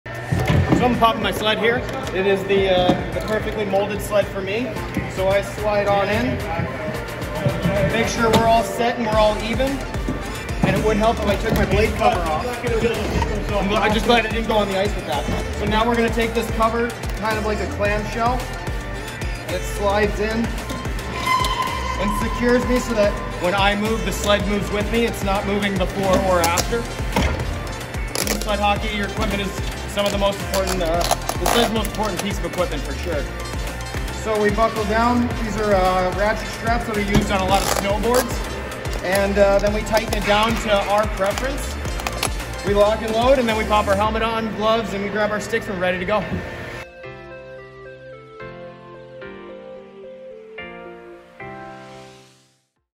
Intro to Sled Hockey | Josh Pauls Explains the Sled
Josh Pauls of the 2022 U.S. Paralympic Sled Hockey Team demonstrates getting into his sled and explains what makes each sled unique.